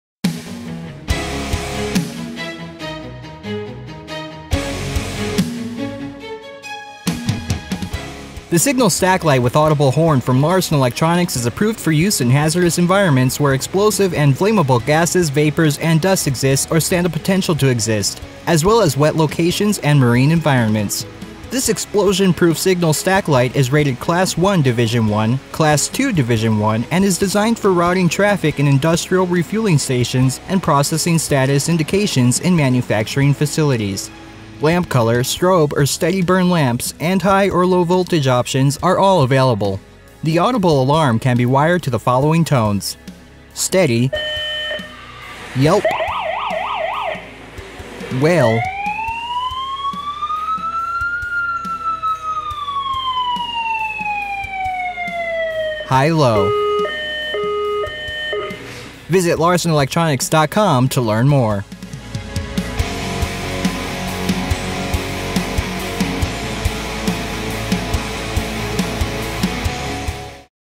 Explosion Proof Signal Light w/ Audible Alarm - Class 1 Div 1 Class 2 Div 1 - Multiple Colors
The EPL-TL-1X10W-C-PA Class 1 Division 1 and Class 2 Division 1 LED Signal Light with Audible Horn from Larson Electronics is approved for use in hazardous environments where explosive and flammable gases, vapors, and dusts exist or stand a potential to exist, as well as wet locations and marine environments. This explosion proof signal stack light light is designed for routing traffic in industrial refueling stations and processing status indications in manufacturing facilities.

The EPL-TL-1X10W-C-PA explosion proof signal stack LED light with audible horn an signal/traffic light that offers 10 Watts of LED light in red, green, amber, blue, white, or purple light output. This unit is rated for Class 1 Division 1 and Class 2 Division 1 work environments, T3 temperature rated, weatherproof/dust-proof, and NEMA 4X and marine rated for wet environments. These indicator lights have 50,000 hours of operational life and the unit works on 120/240 volts AC or 24 volts DC configurations for standard or low voltage operation.

This explosion proof fixture is configured for red, green, amber, blue, white, or purple output and offers exceptional 1050 lumen output each. The explosion proof audible alarm setup sounds a unique tone per the user setting. Multiple units can be configured for different lamp colors and different tones, providing operators in hazardous locations with an effective and powerful alert/signal system that can withstand demanding conditions and be heard over the operation of industrial machinery and equipment. A NEMA 4X rated explosion proof junction box protects the internal wiring and can be tapped in order to connect this stack light to power sources. This explosion proof signal light is durable, can operate in any weather condition, and is rated for Class 1 Division 1 and Class 2 Division 2 areas where flammable gas and dust are present or have potential to exist.

The 10 watt lamp in this hazardous location stack light provides high output in visible signal colored LED lamp, with a lifespan of 50,000 hours, is protected by durable Pyrex globes and cast aluminum globe guards for added durability, and offer cool operation and clean, bright output surpassing that of 100 Watt incandescent indicator lamps. The LED lamp is colored LEDs instead of colored globes, providing more light output. We offer strobe or steady burn lamps for this fixture. The light housing is constructed of a polyester powder coated, copper free aluminum casting that can withstand 1490 pounds PSI hydrostatic pressure. The housing on this explosion and water proof proof stack light is constructed of heavy duty aluminum while the horn is made of fiberglass. The entire unit is finished in a durable gray powder coat for resistance to corrosion.

This stack light is equipped an explosion proof horn. Attached to the far end of the fixture, this audible horn provides operators with up to 5 different tones that can reach a peak output of 110dBA, powerful enough to be heard over industrial operations. There is no dimming or volume control for the lights or horns.

Wiring is through a 3/4" NPT conduit access hub. The unit can be ordered in 120V or 240V AC configuration for use with standard line voltages or 24V DC for low voltage use. Mounting is provided by a heavy gauge aluminum base with predrilled holes designed to be bolted to walls and flat surfaces for permanent and secure attachment. Power is provided by individual conductors to allow independent operation of each lamp and tone combination. Operators can tap the C1D1 junction box in order to connect to a power source.

This 10 Watt single lamp explosion proof signal stack LED light with alarm system is an ideal fixture indicator for manufacturing facilities, routing shipping lanes, and any hazardous work area where status indicators and audible alarms are necessary.